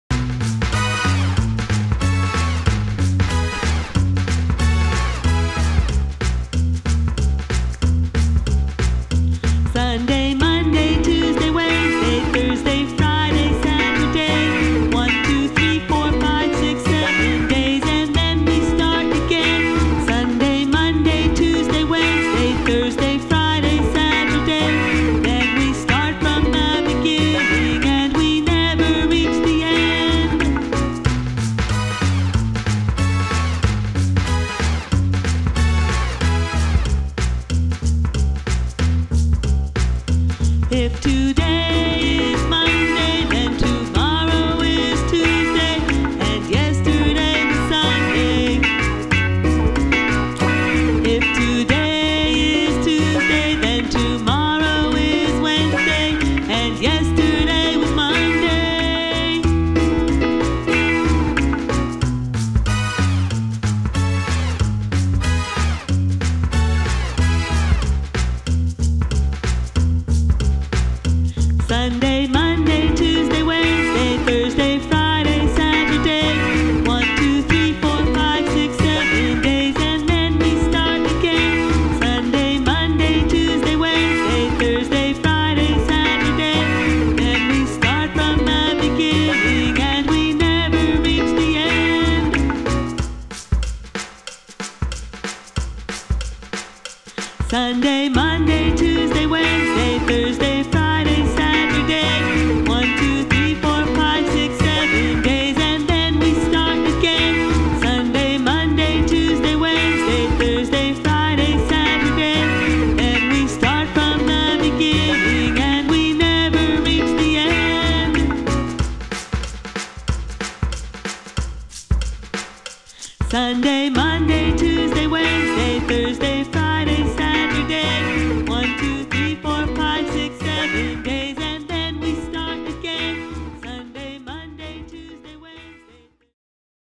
Days of the Week
A fun way to learn the days of the week.

רוצים עוד? כנסו לאתר "לומדים אנגלית בבית בכיף"

Looking for more activities? Check out my online course "Teach English vocabulary without a textbook"